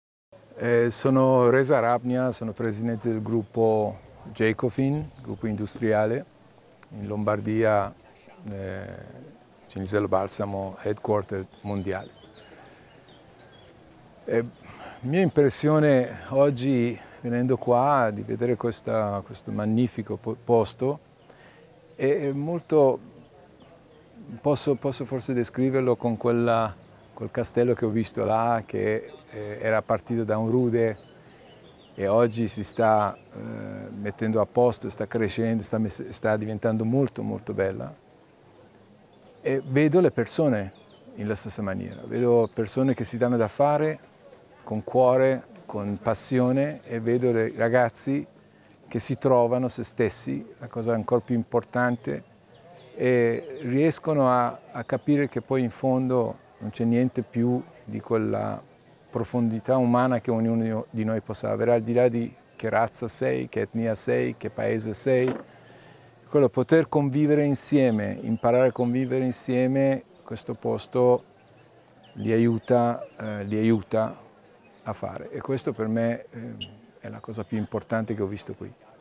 Ali Reza Arabnia, Chairman e CEO gruppo Gecofin Spa a YoutopicFest23
Ali Reza Arabnia, Chairman e CEO gruppo Gecofin Spa a YoutopicFest23, il Festival Internazionale sul Conflitto di Rondine a Rondine Cittadella della Pace (Arezzo)